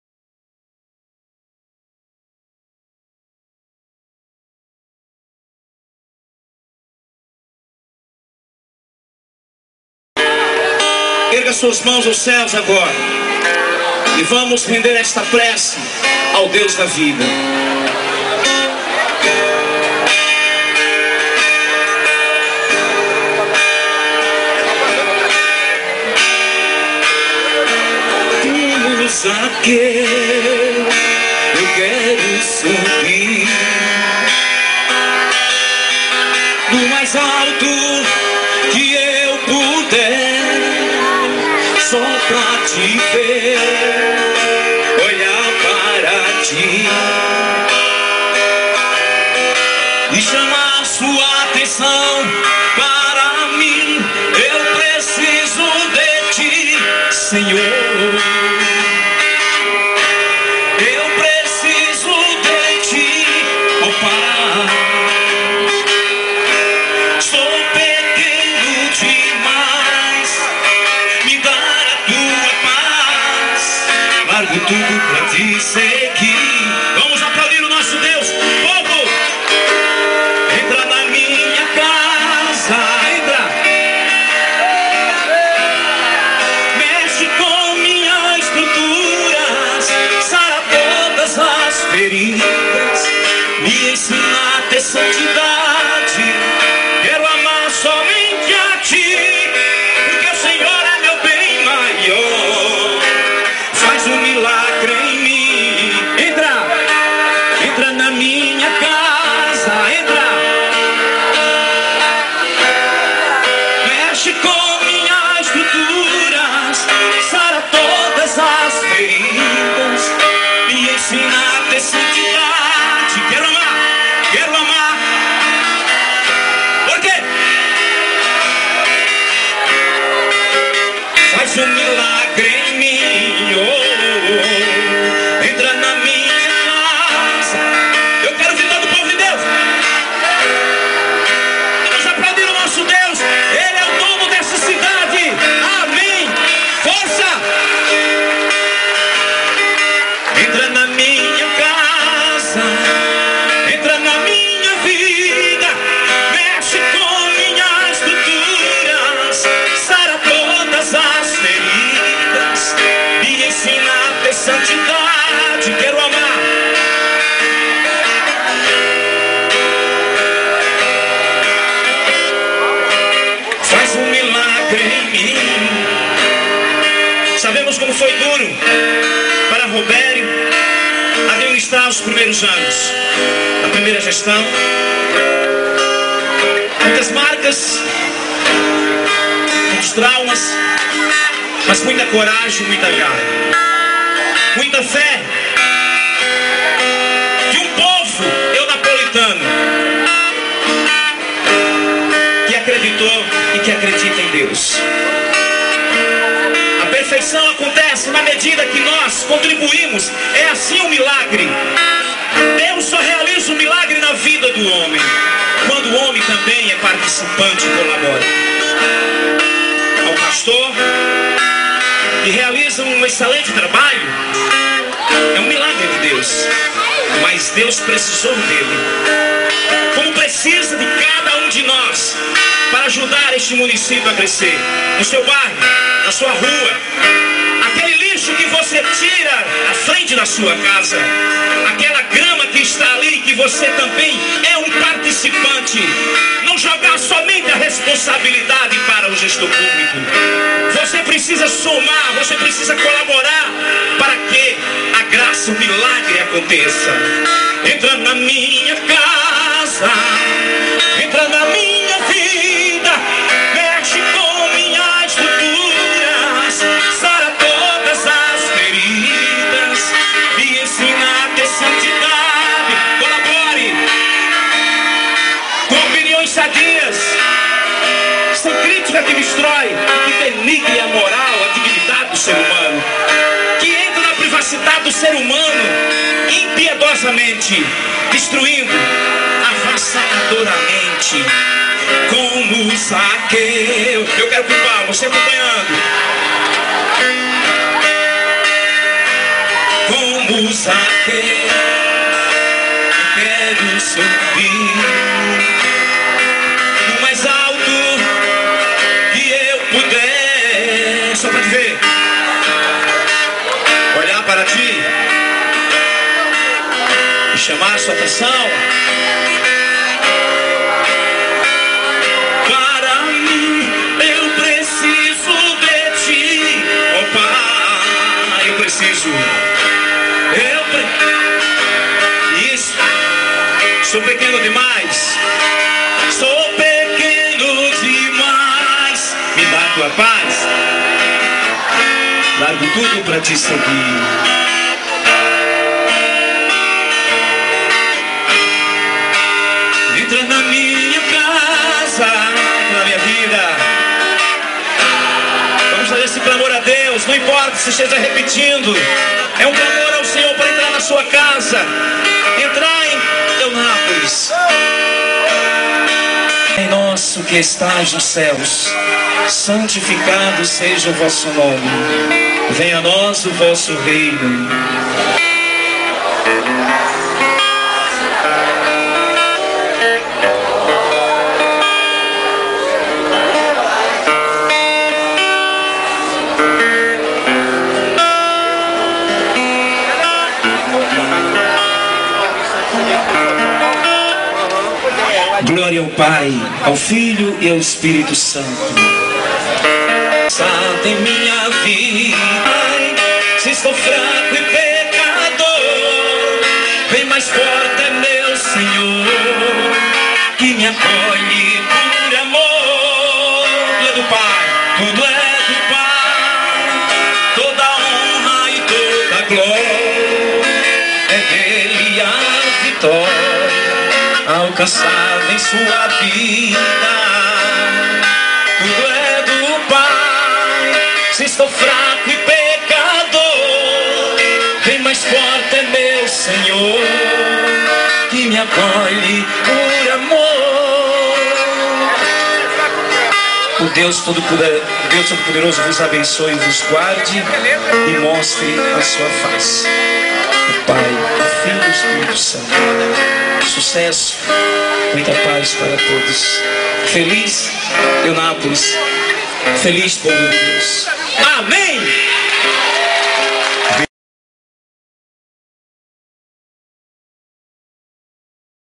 Filme_Momentos com Deus 22 Anos_Eunapolis.flv
Manifestação religiosa pela Igraja Católica nos festejos de emancipação de Eunápolis